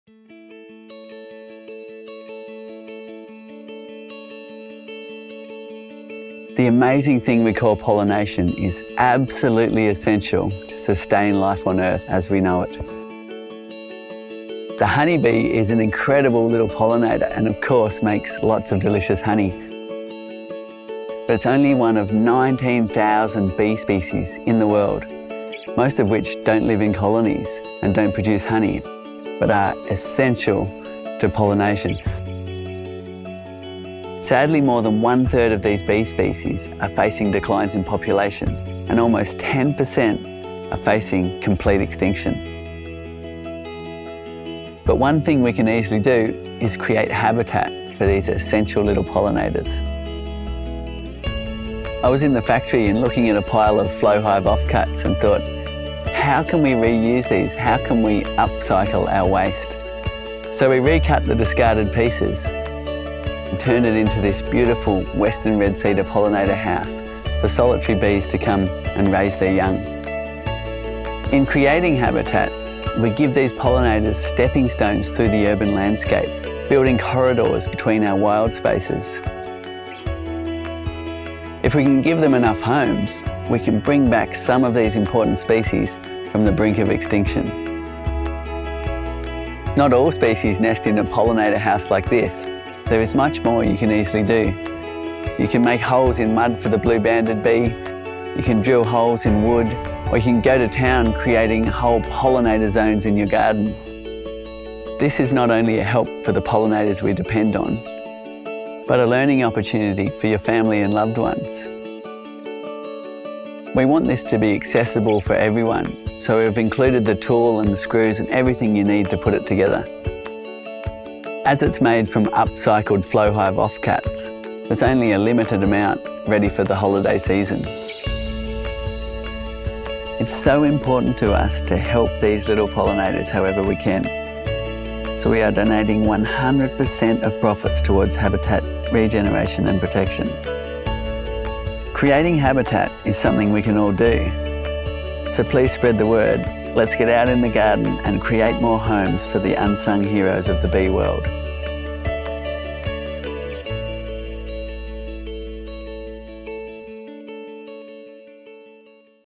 Flow Pollinator House is a cosy home for solitary pollinators
The honeybee is one of 19,000 bee species in the world and together they’re critical for the amazing thing we call pollination.

Tragically habitat loss and pesticide use, means 37% of our bee species have declining populations, and almost 10% are facing complete extinction. That’s a really unimaginable statistic.

But we can all do something to change this. By creating habitat we’ll give these essential pollinators stepping stones through the urban landscape; building corridors between our wild spaces.

The Flow Pollinator House provides a cosy home for solitary pollinators in your garden, and supports habitat protection at the same time.